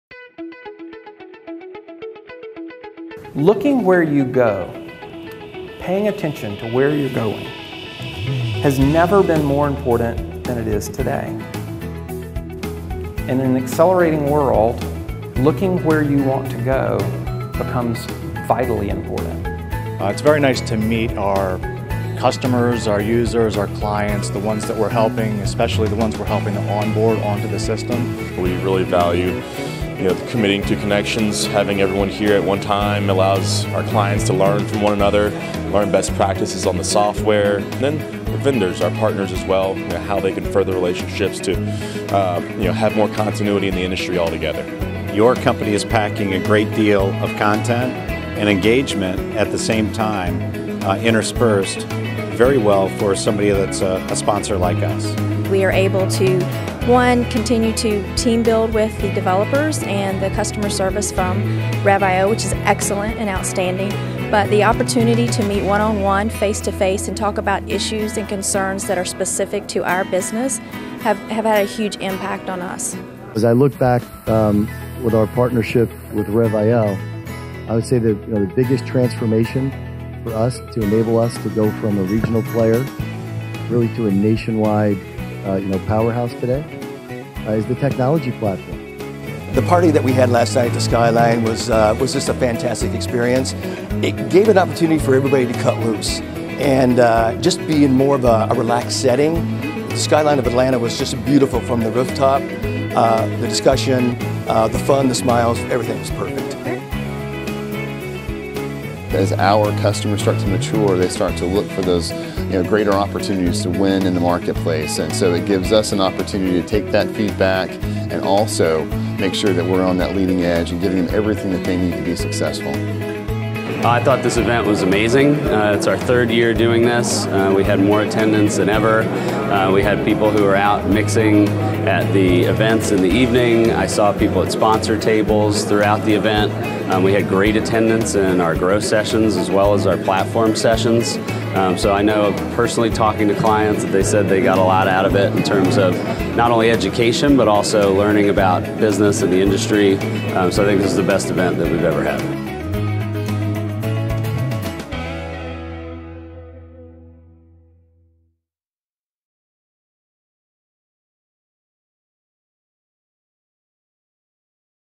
Rev.io Client Summit 2016
Rev.io's 3rd annual Client Summit took place from Monday, September 19 through Wednesday, September 21, 2016 in Atlanta, Georgia.  Companies converged from all over the country to hear from industry leaders, leverage the Rev.io community, and learn from peers and Rev.io experts how to transform business as we know it.